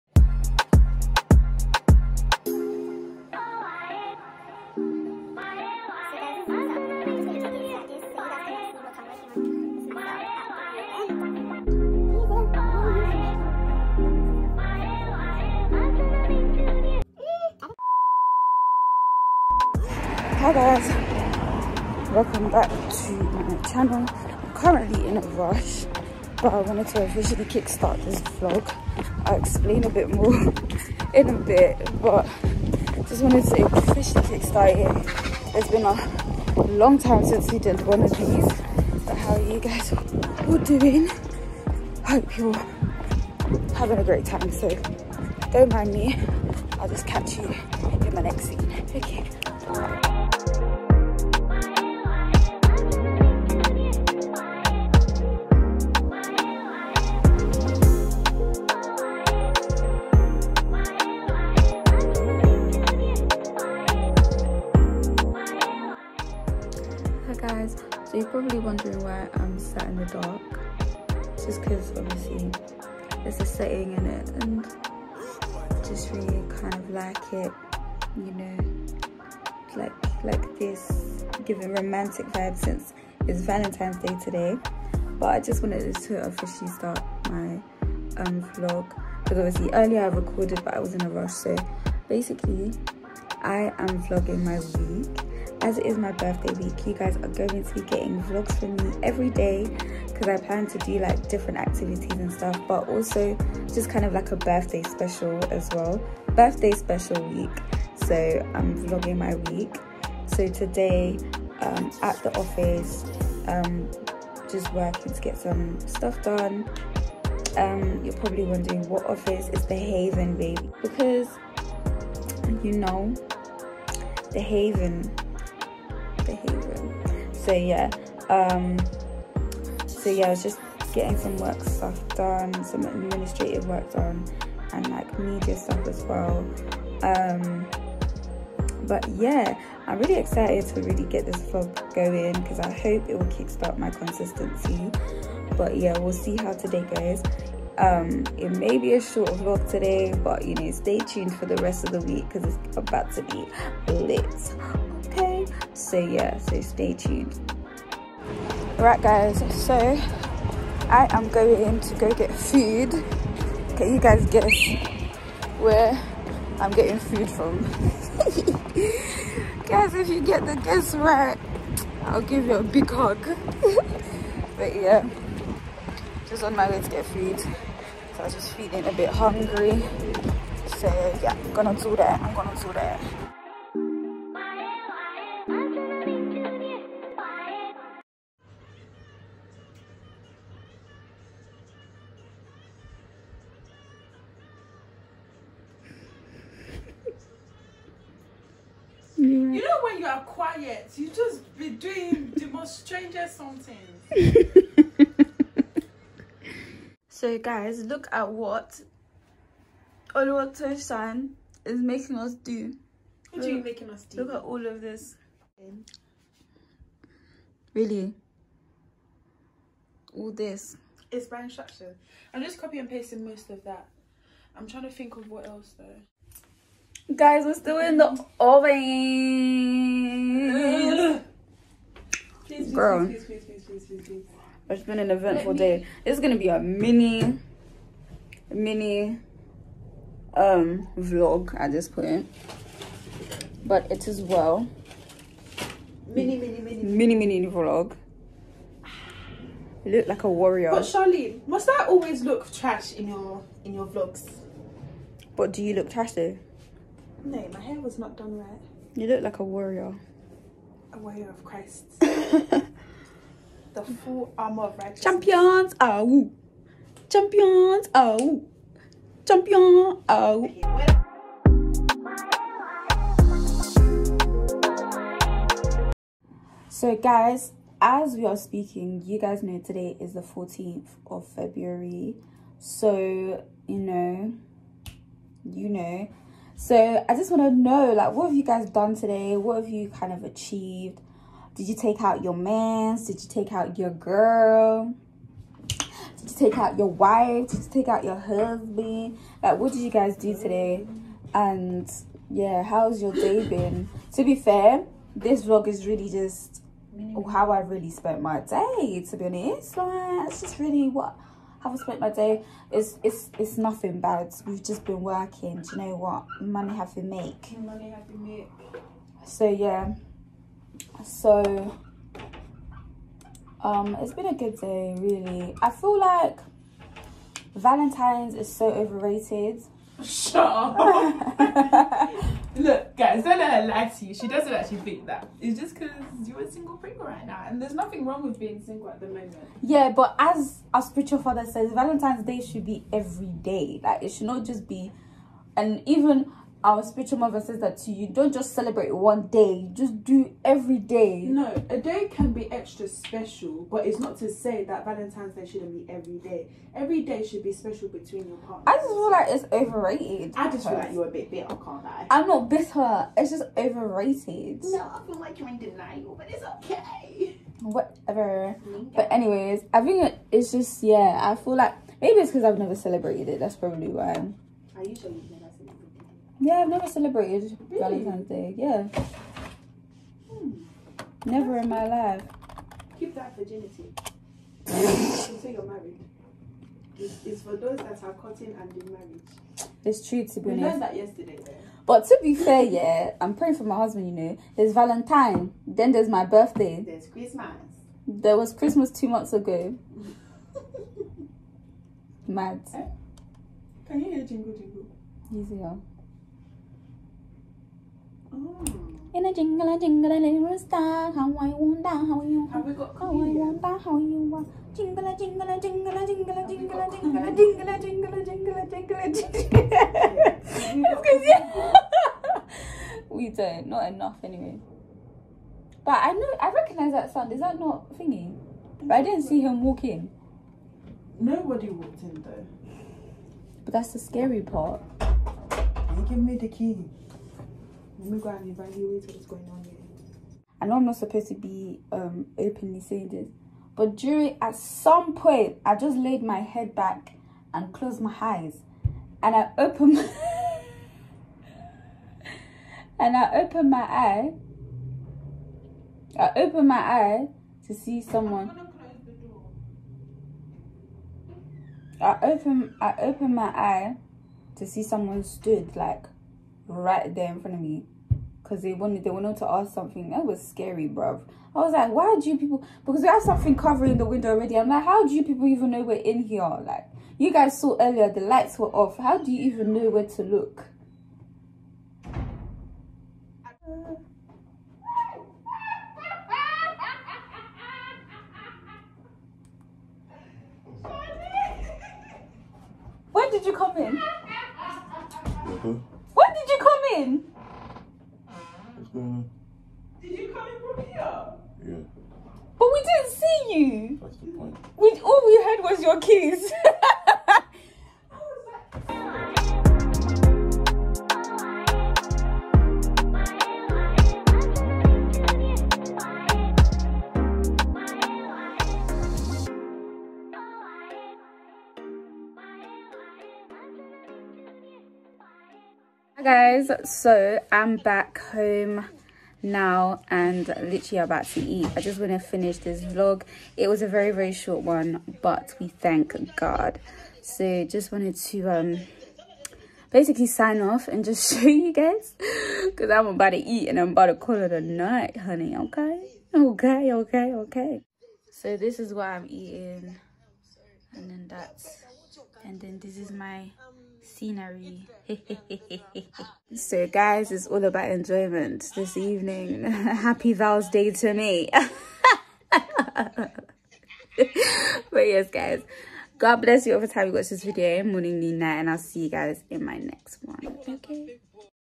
BIRTHDAY WEEK SPECIAL | Day in the Life Working a 9-5 Office Job | DAY 1 | 14.02.22
Hey guys!

Just to kick start my Birthday Week, I show you all me working in the office and just a nice day. It is a short Vlog the day was fabulous.